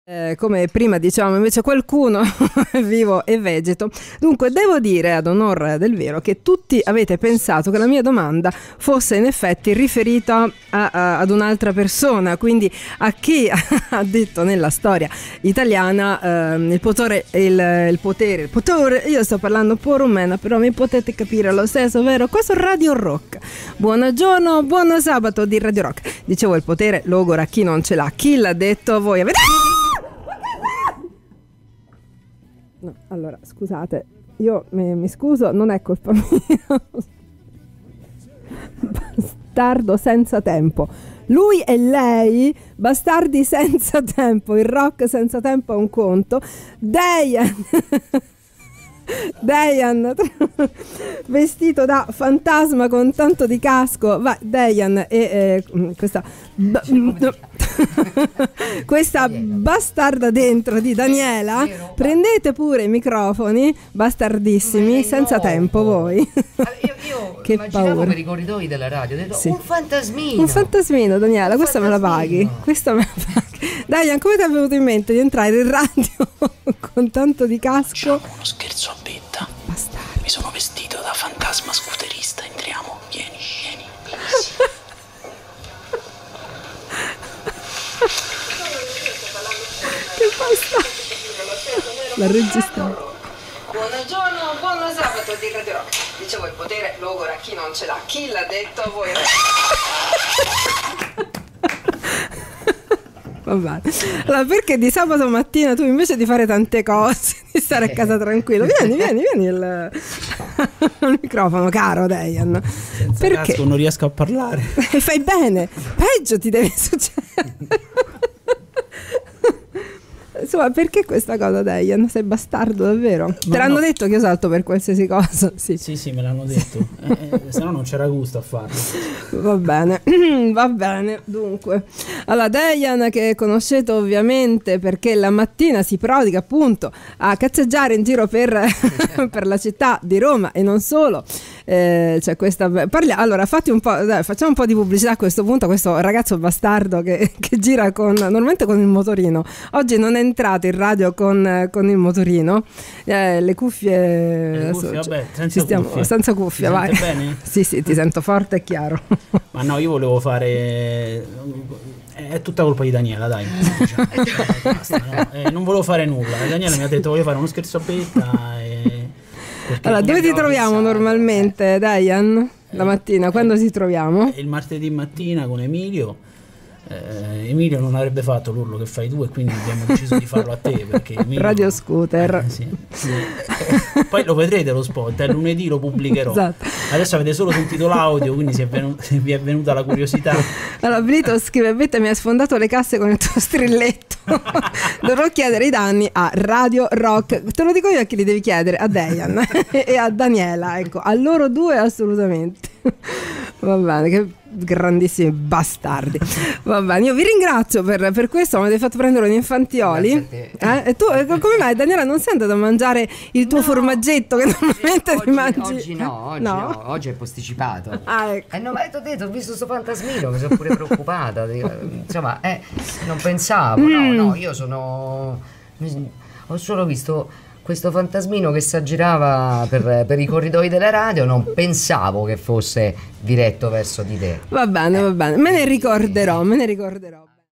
[RADIO ROCK] Lo Scherzo dello Scooterista Fantasma a Betta Cianchini
Con la complicità di DanyRock, Dejan spaventa Betta Cianchini mentre è in diretta su Radio Rock